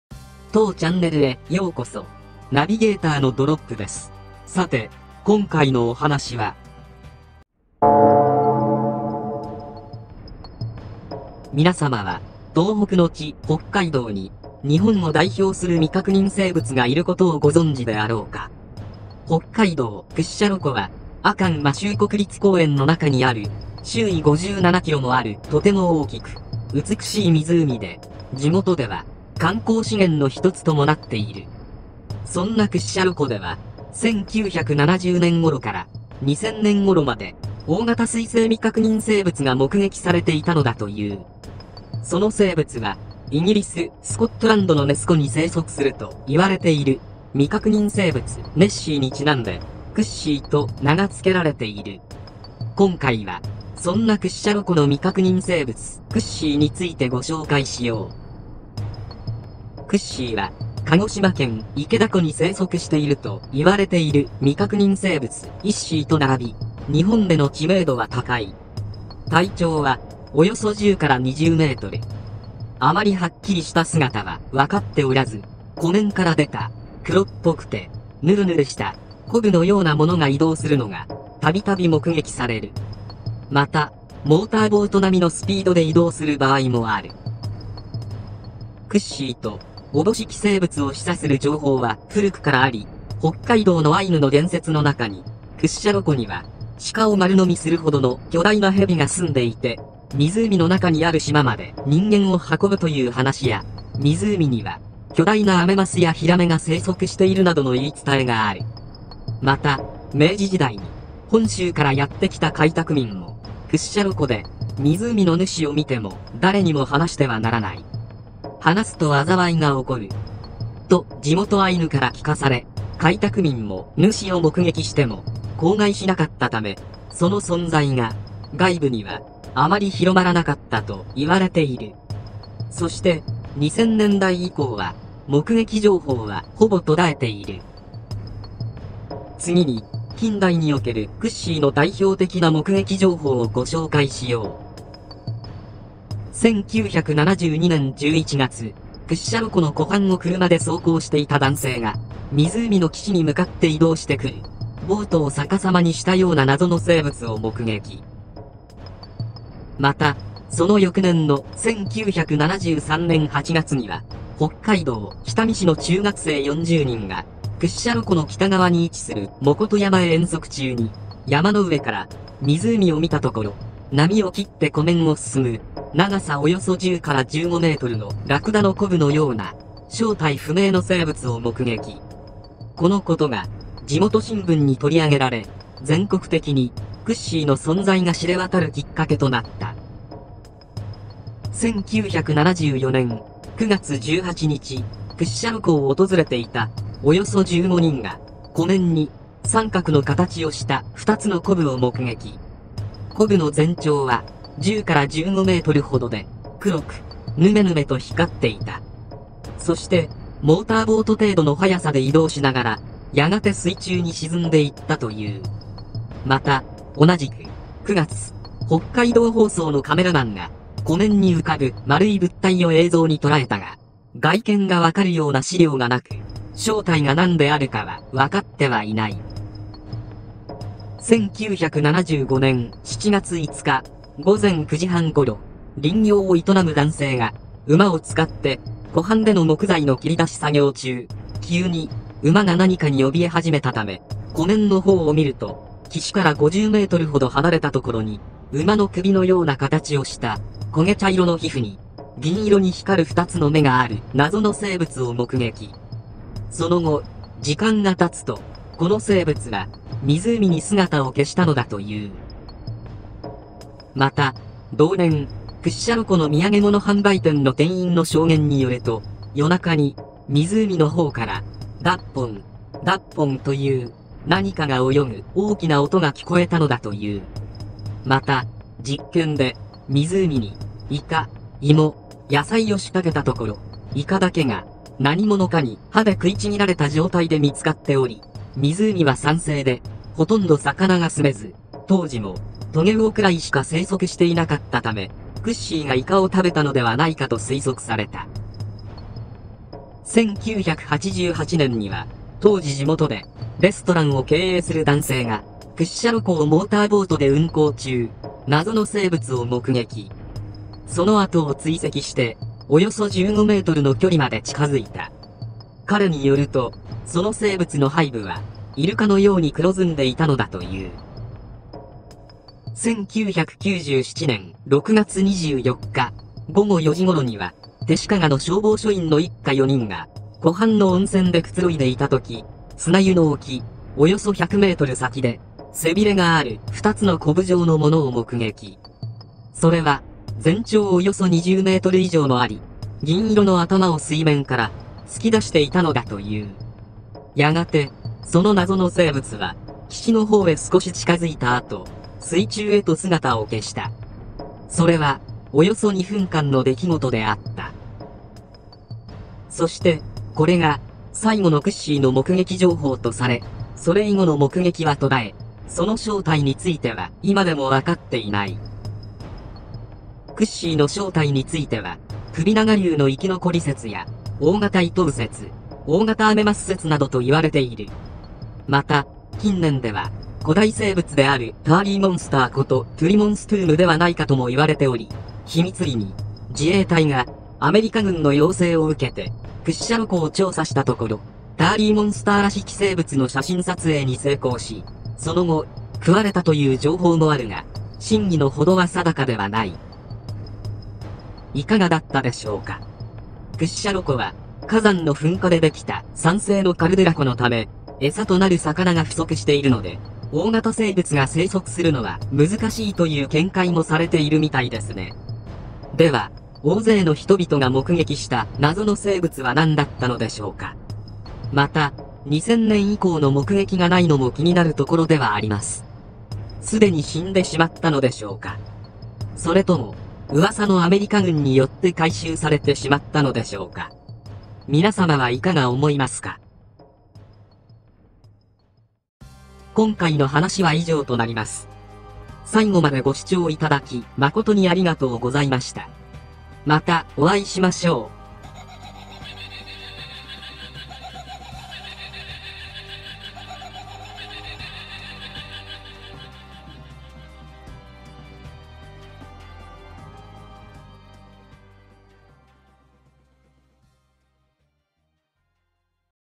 アイヌが語り継いだ伝説の水棲獣 クッシー
東北の地 北海道に、日本を代表する未確認生物がいる事をご存知であろうか。

北海道 屈斜路湖は、1970年頃から、2000年頃まで、
大型水棲未確認生物が目撃されていたのだという。
その生物は、イギリス スコットランドのネス湖に生息すると言われている、
未確認生物 ネッシーに因んで、「クッシー」と名が付けられている。
今回はそんな屈斜路湖の未確認生物 「クッシー」についてご紹介しよう。